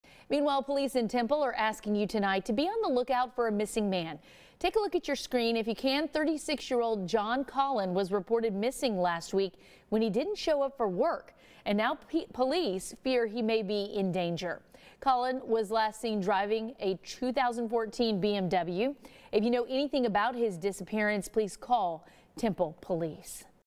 Temple police looking for missing man
John Cain has not reported to work for the last few days, according to police.